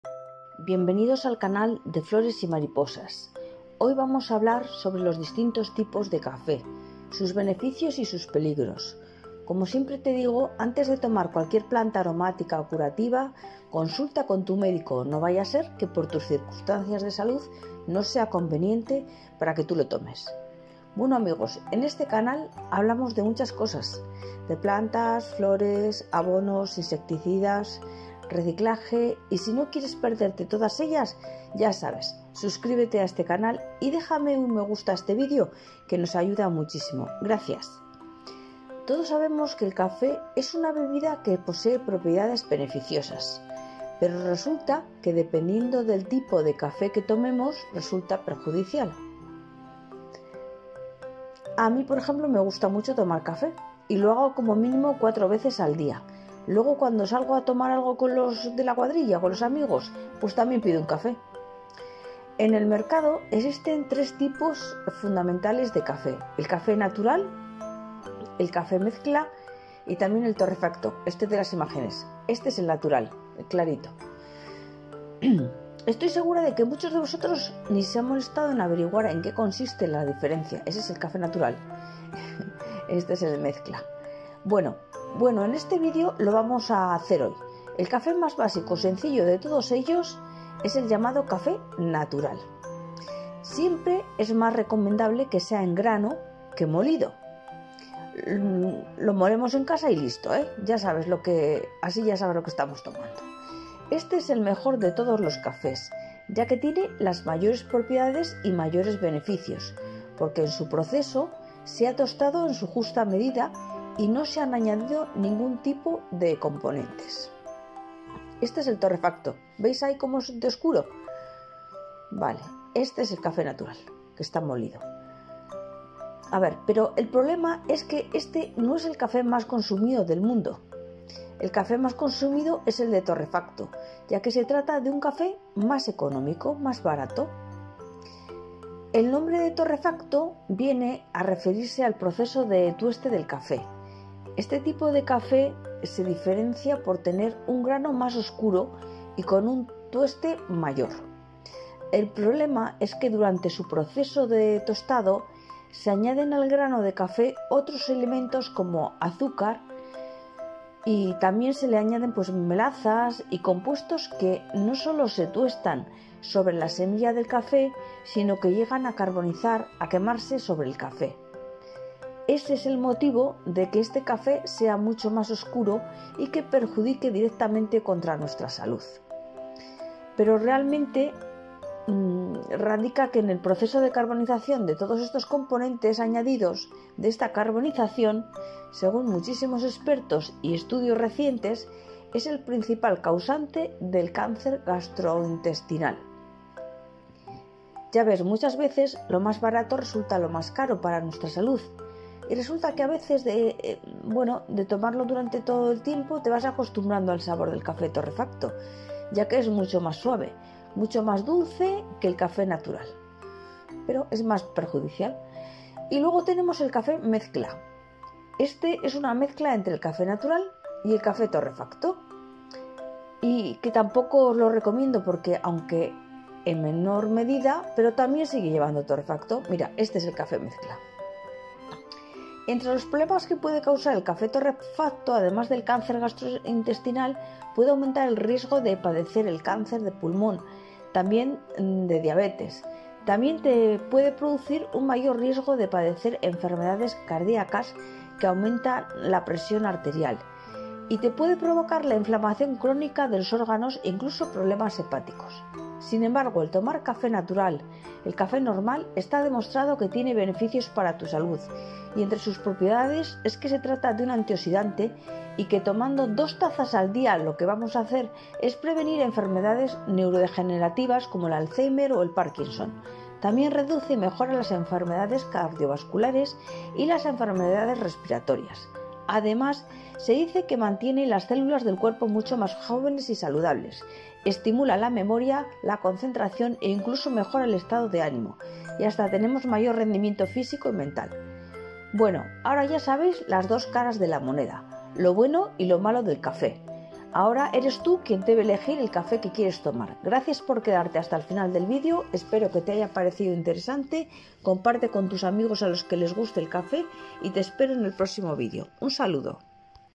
Los IMPACTOS del CAFÉ en tu Salud: Lo que Sucede en tu CUERPO con su Consumo DIARIO☕☕☕
Dos tazas de café al día, es una medida perfecta para tu salud.